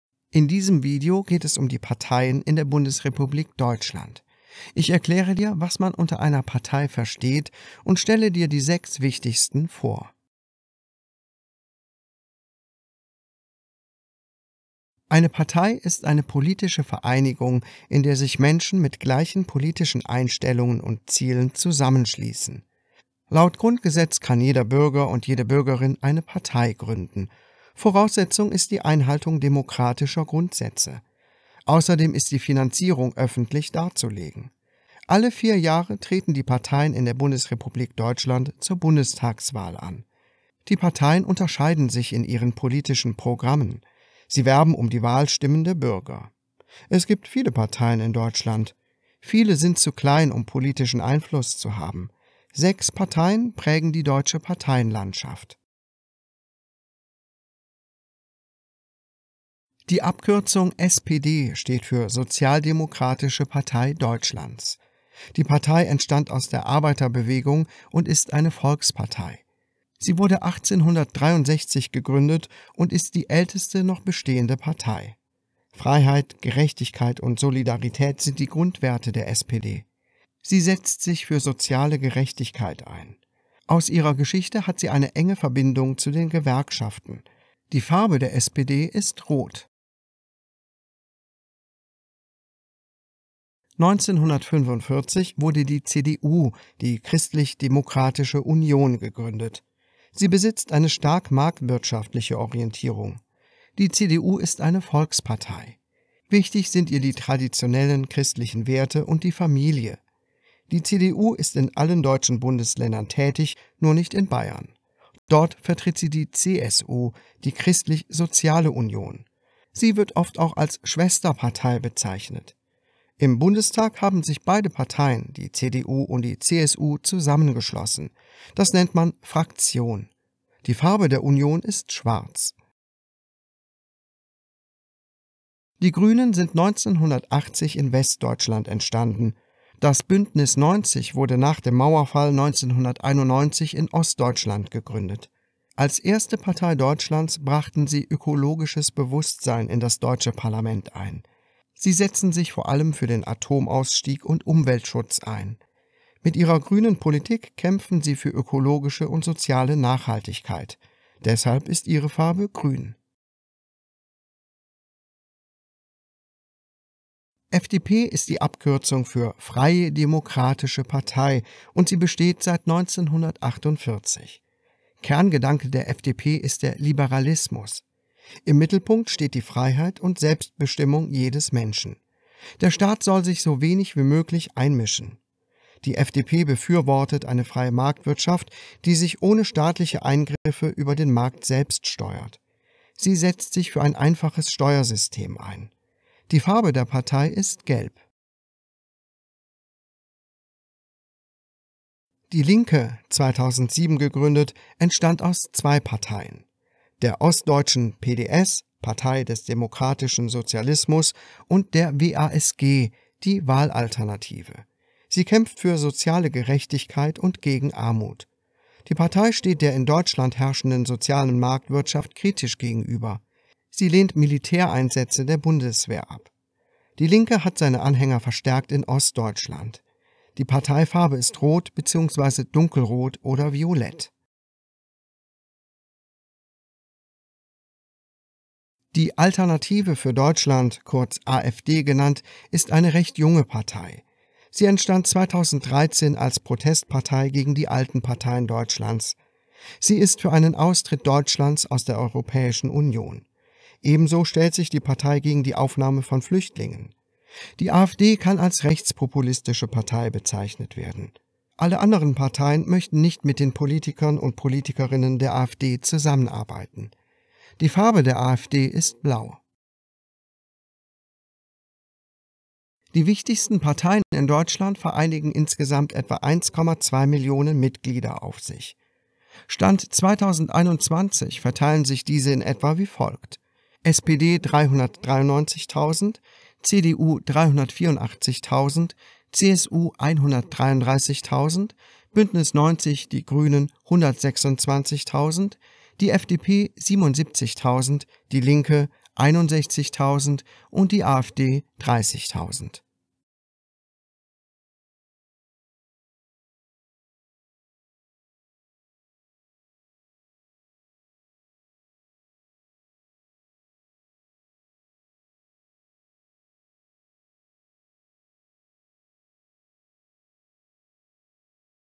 Die Parteien in Deutschland einfach und kurz erklärt
Erfahre in weniger als 6 Minuten, was du über die Parteien in Deutschland wissen solltest!